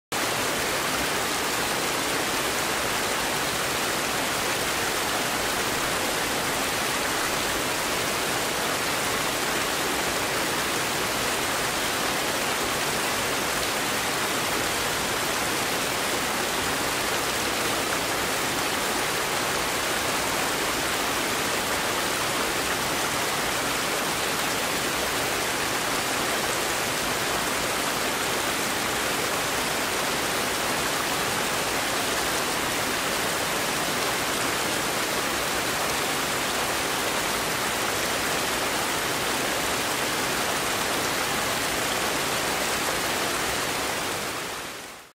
River raging in forest, Nature Video Stock Footage_0250
Nature Video Stock Footage of raging river in forest, filmed in High Definition.